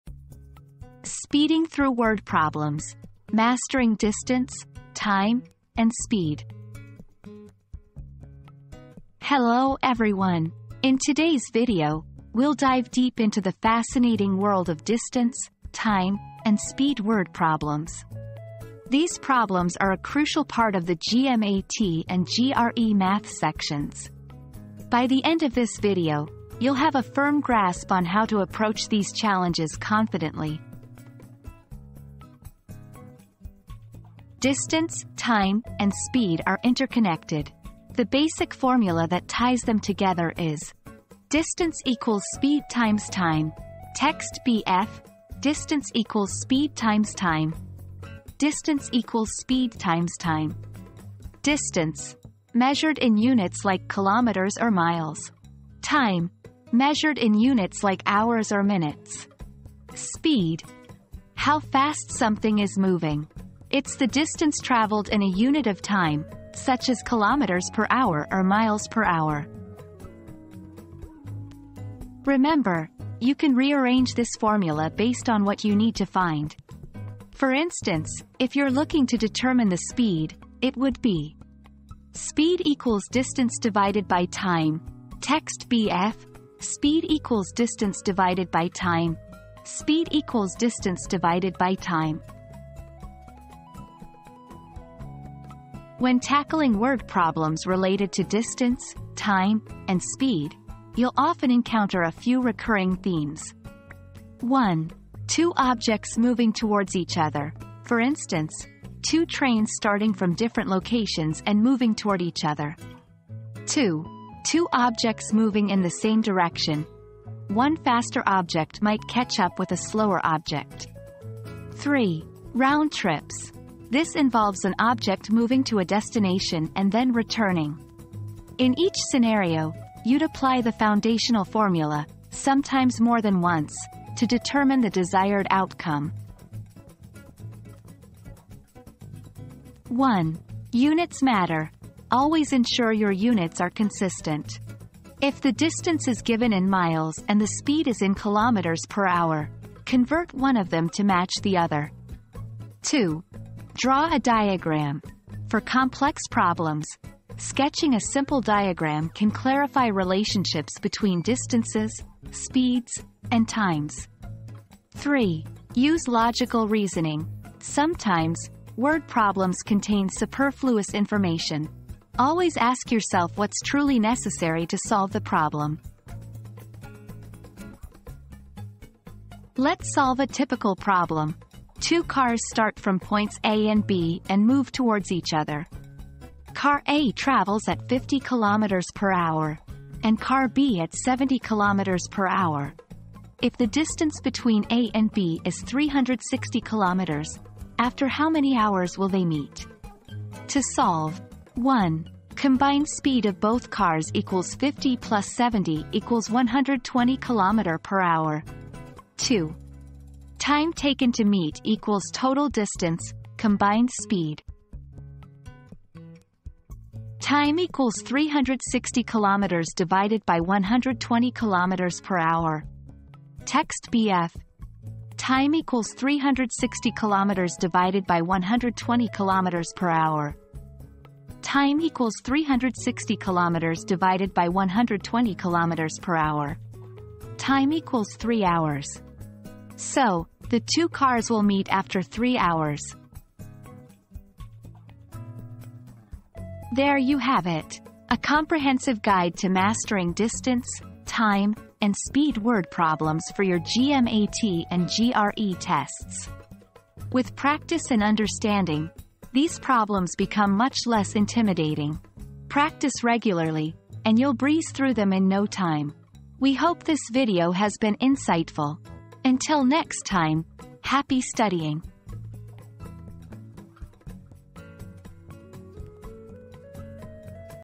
Speeding Through Word Problems: Mastering Distance, Time, and Speed
Mastering Distance, Time, and Speed: Speeding Through Word Problems • Learn how to confidently approach distance, time, and speed word problems in this informative video. Understand the basic formula and gain the skills to solve these challenges effectively.

00:00 • Introduction - Speeding Through Word Problems: Mastering Distance, Time, and Speed
00:33 • Understanding the Basics
01:35 • Common Scenarios in Word Problems
02:19 • Tips and Tricks
02:59 • Sample Problem